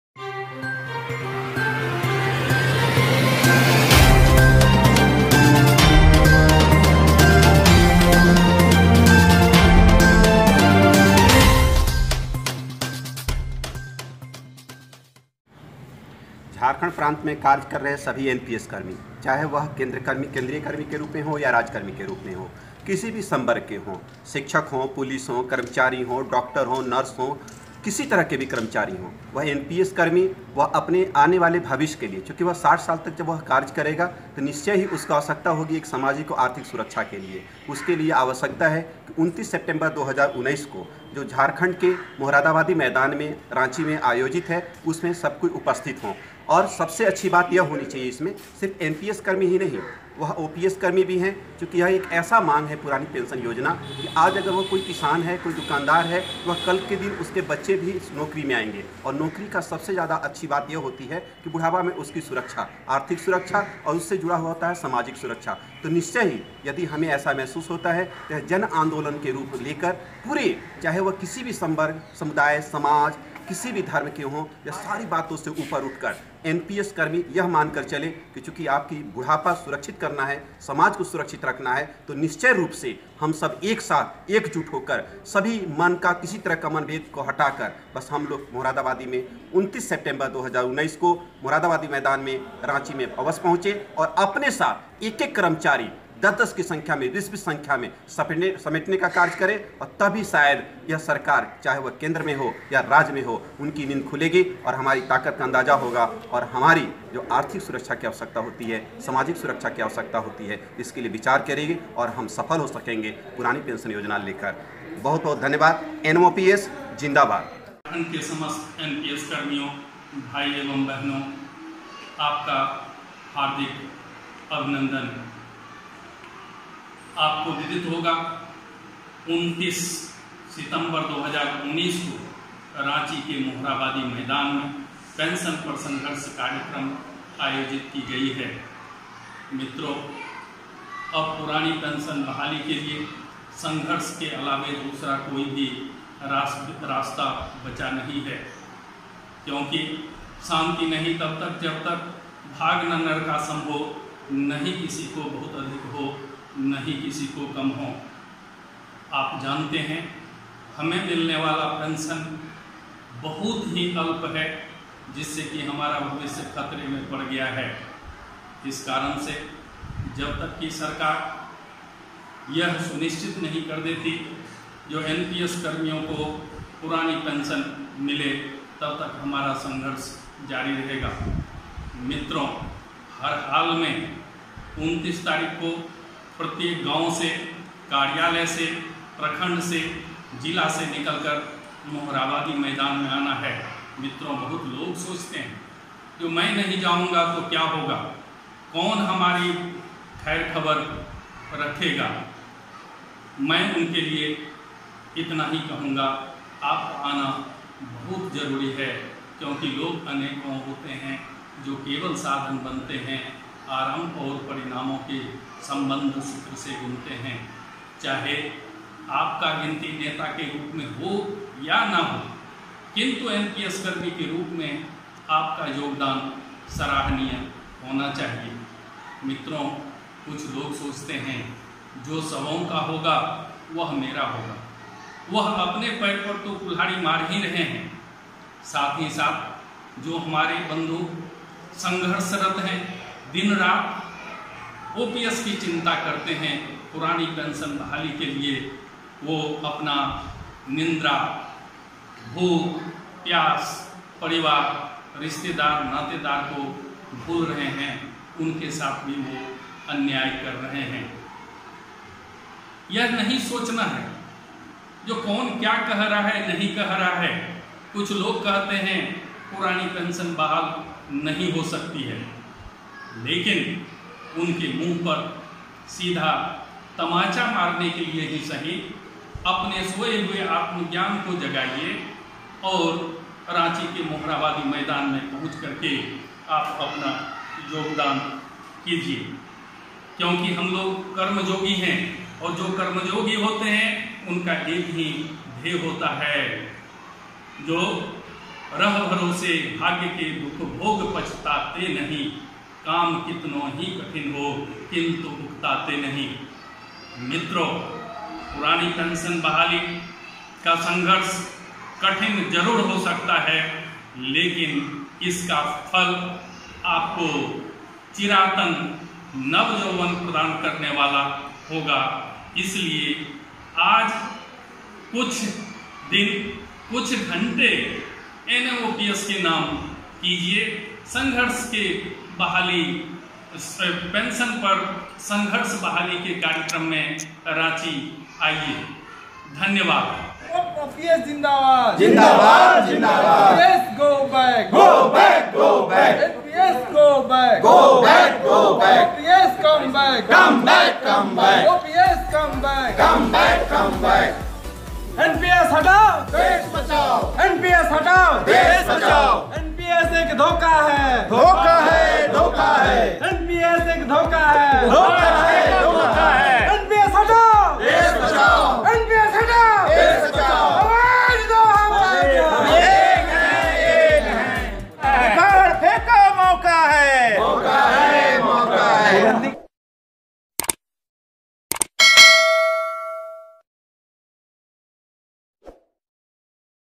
PURANI PENSION BAHALI-पेंशन पर संघर्ष के लिए किया अपील, PART - 1
पुरानी पेंशन बहाली के लिए 29 सितंबर 2019 को रांची के मोराबादी मैदान से मुख्यमंत्री आवास का घेराव करने के लिए पुरानी पेंशन बहाली राष्ट्रीय आंदोलन झारखंड ने किया सभी एनपीएस कर्मियों को अपील